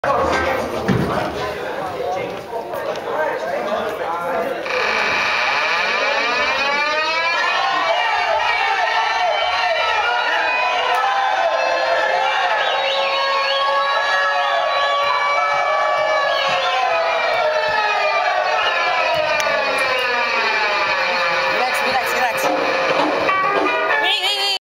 Crickets To Jackhammers *noob*
Crickets to jacks, this was a pretty long time ago when i cant still do it from toprocks to crickets
i use my knee to go down XD i guess that was a pretty good idea but oh well i can do top to crickets now :)

ill upload more vids of me soon XD and my battles, if i can get them from ppl who took a vid of me :)